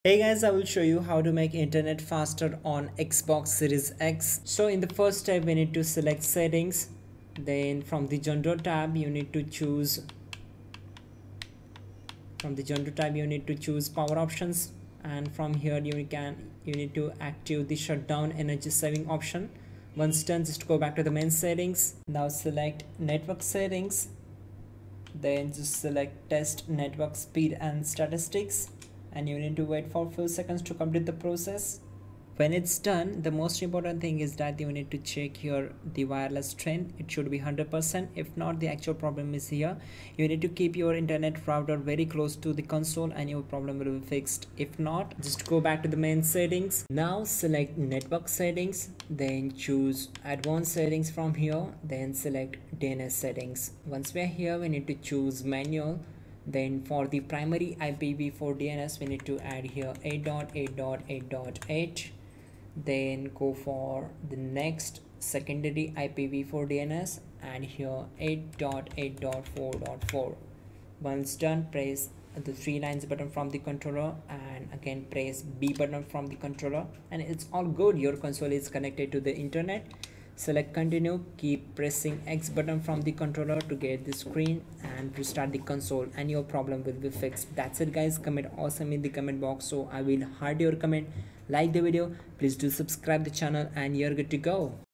How to Make Internet Faster on Xbox Series X
To enhance your gaming experience, knowing how to make internet faster on Xbox Series X is crucial. Firstly, ensure your console is connected to a high-speed internet connection. Wired connections generally offer better speeds and stability compared to wireless ones, so consider using an Ethernet cable. Additionally, close any background apps or games that might be using bandwidth. Another tip is to prioritize your Xbox's network traffic. Most routers allow you to prioritize certain devices or applications, ensuring your console gets the necessary bandwidth for gaming. Updating your router's firmware and optimizing its settings can also improve internet speed. Lastly, if possible, try to reduce interference from other wireless devices by placing your router in a central location and away from obstacles. By following these steps, you can optimize your Xbox Series X's internet connection for smoother gaming sessions. How to Make Internet Faster on Xbox Series X